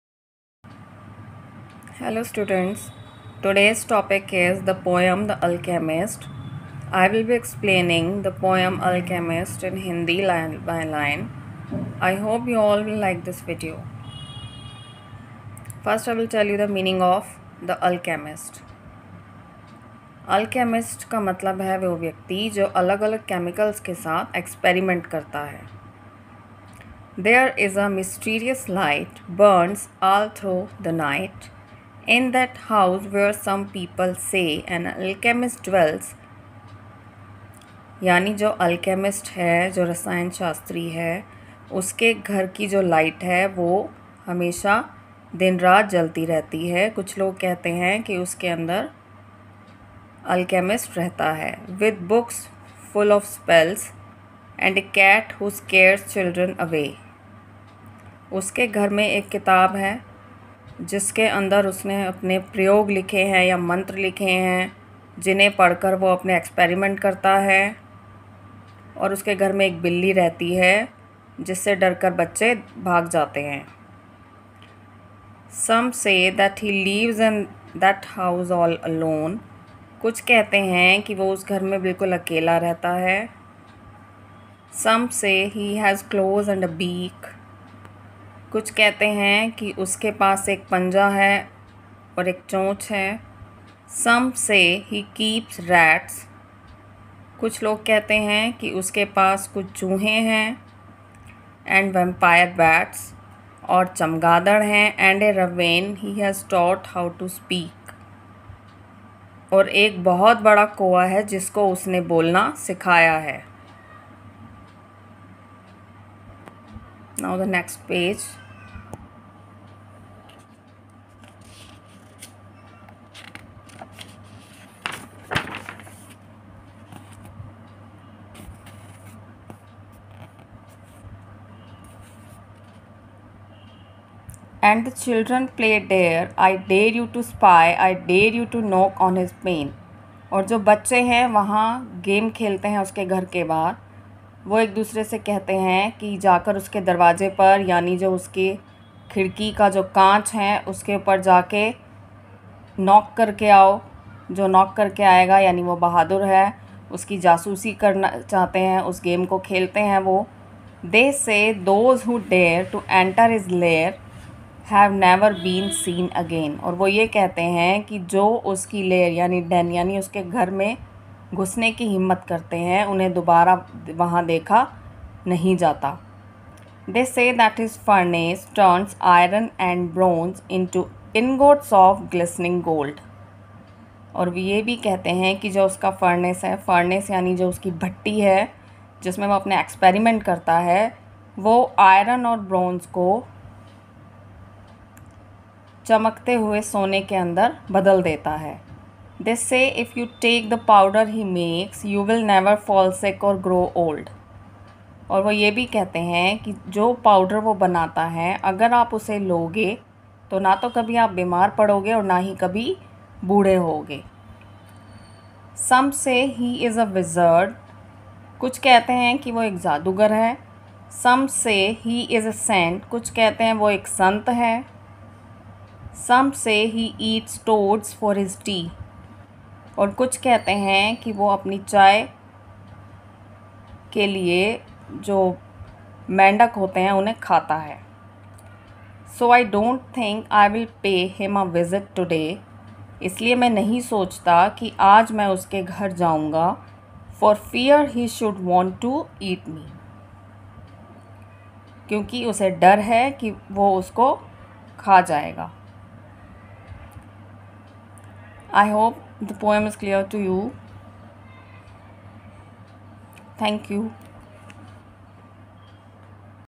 English||Class 6|| Poem The Alchemist|| Line by line Explanation
I will be explaining poem The Alchemist